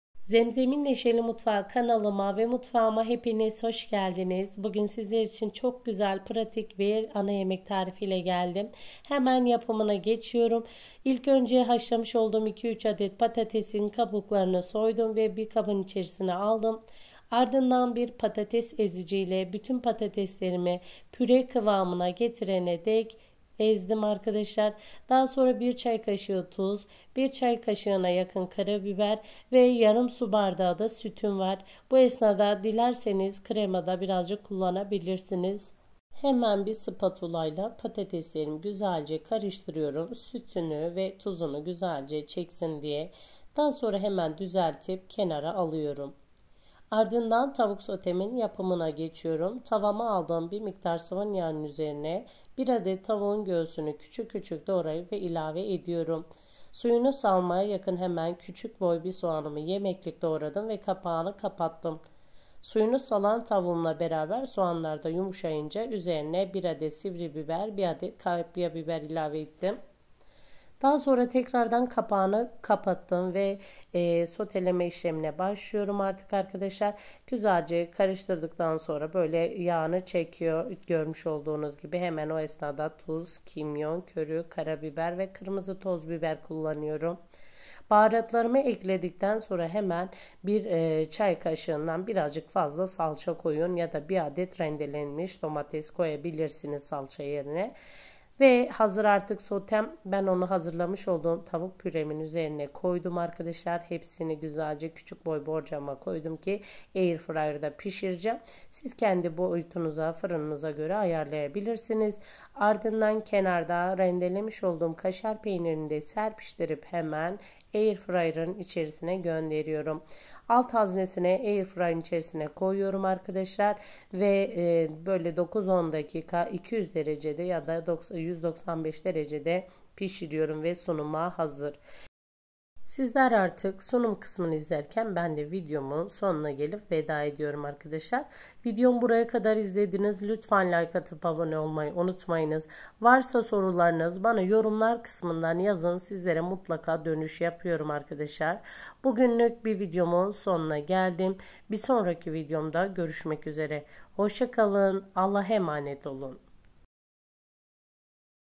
Efsane Bir Yemek Airfryer'da Patatesli Tavuk Sote✅Airfryer'da Yemek Tarifleri✅Patates Püreli Sote
MALZEMELERİ
PÜRE İÇİN
3-4  adet patates
yarım su bardağı süt
tuz
kara biber
SOTE İÇİN
1 adet tavuk göğsü
1 adet soğan
1 adet sivri biber
1 adet kapya biber
Tuz
köri
toz kırmızı biber
kimyon
kara biber
1 tatlı kaşığı salça